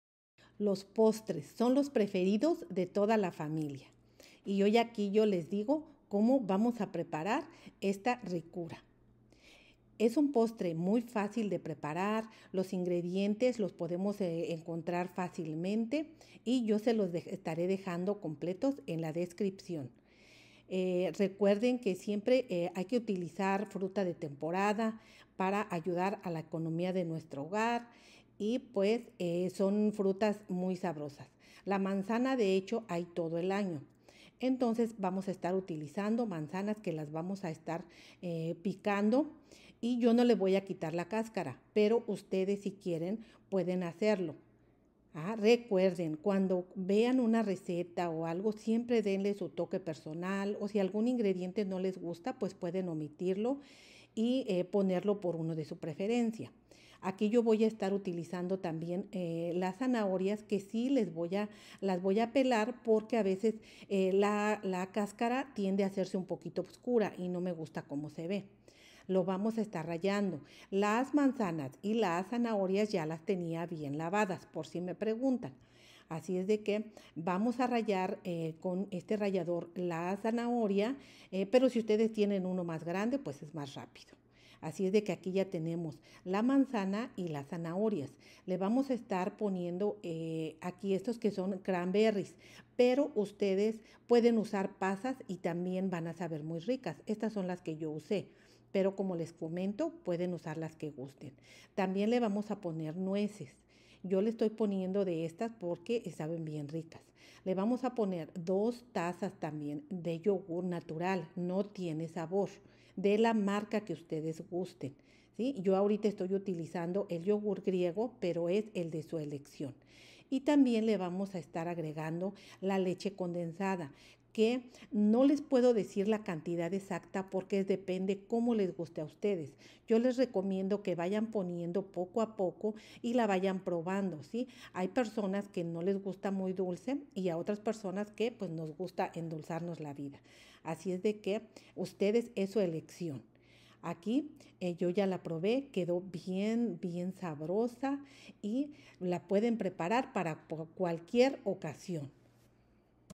Para endulzarnos la vida hoy prepararemos este Suculento Postre de Zanahoria y más delicias!
Tomen nota de este Suculento Postre de Zanahoria y Mas Delicias.

Si te gustan los postres aquí te comparto esta receta.

Ingredientes
5 Zanahorias
4 Manzanas
1 Taza nuez de castilla
1 Taza de arándanos
2Tazas de yogurt griego
1 Taza de leche condensada

Preparación:
Pelar la zanahoria y rallarla, la manzana cortarla en cuadritos
En un recipiente colocar la manzana y la zanahoria rallada, enseguida poner lis demás ingredientes y mezclar muy bien. Servir frío